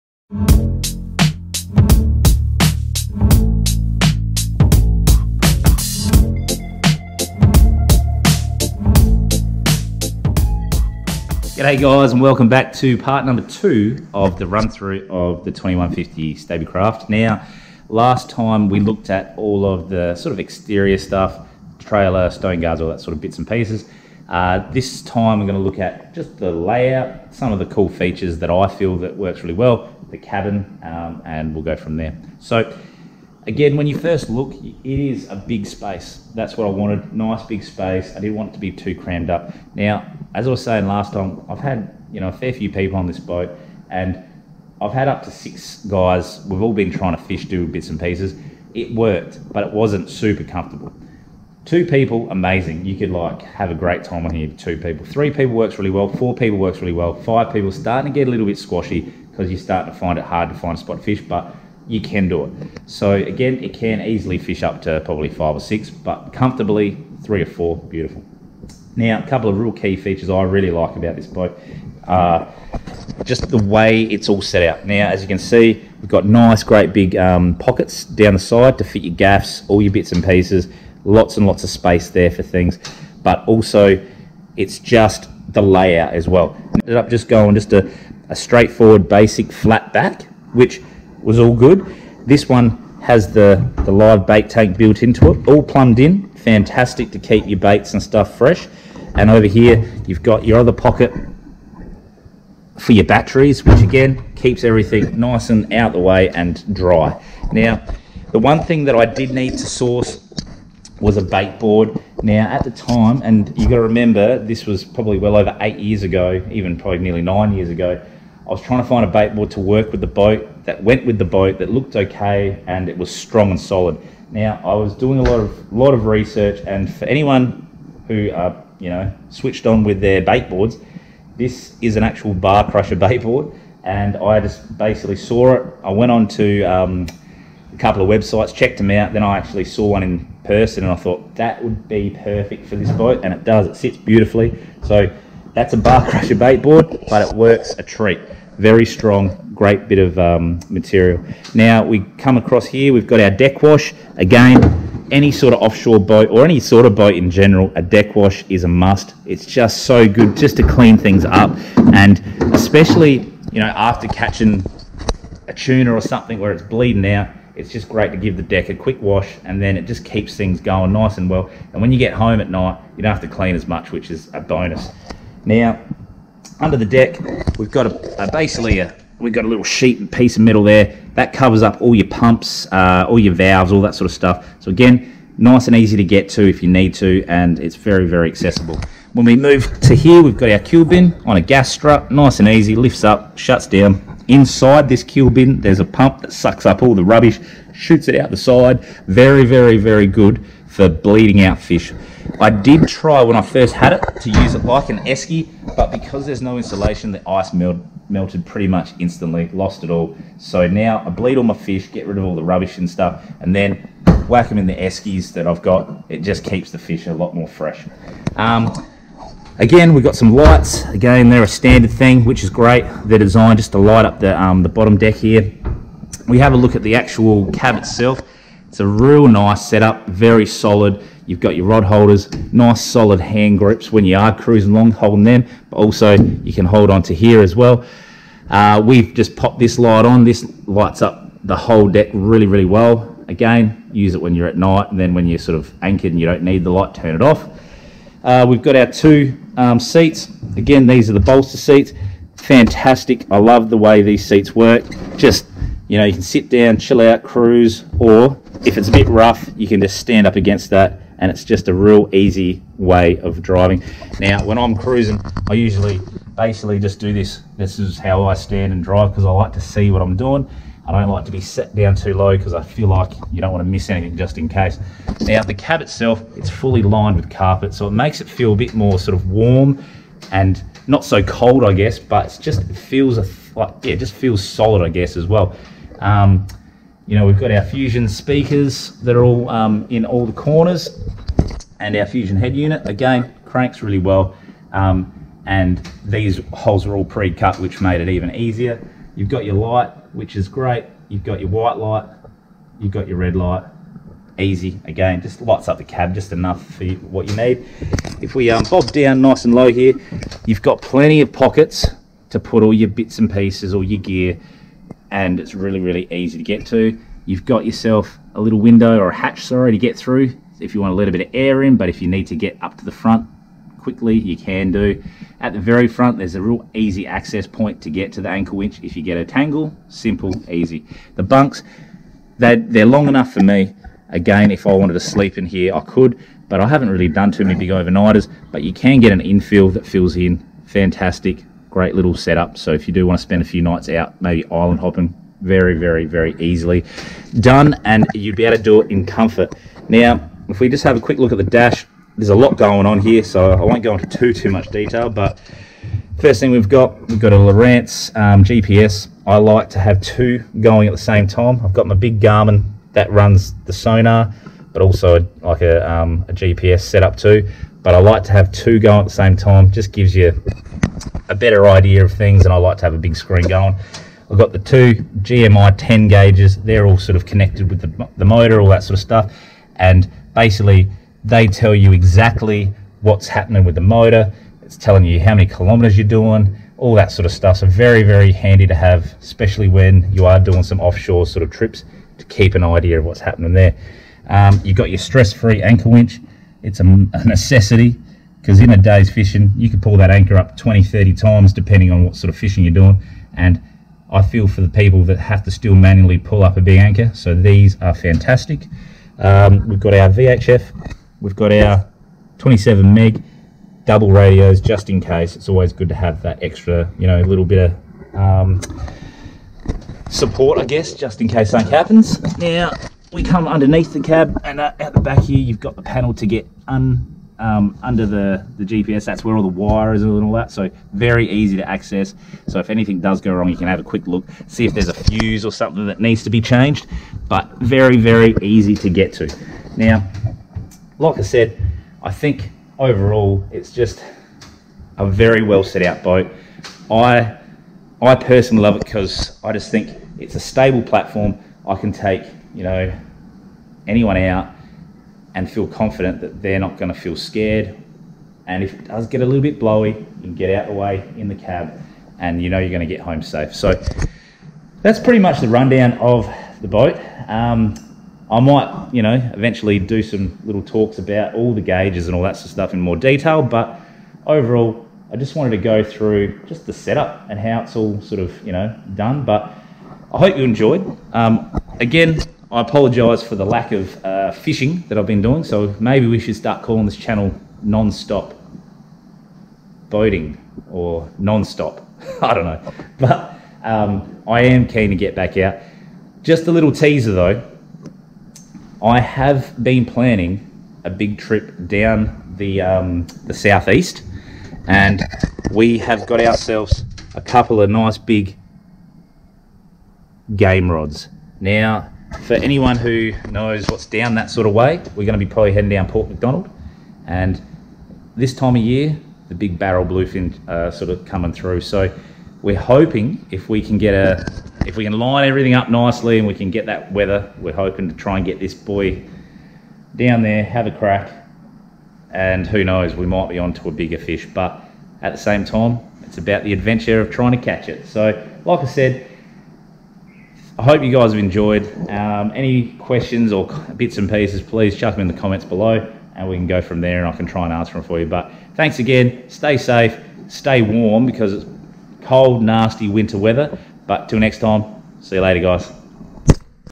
Pt.2/Offshore Fishing Weapon/Layout & Mods/2150 Stabicraft
In this episode we focus on the inside of the big Stabi in part 2. We look at all the great features that has made this boat such a great fishing platform and look at the feature that make day to day use much easier. I feel one of the best feature of this boat is the cab and the layout of it and as you can see it really is built to fish.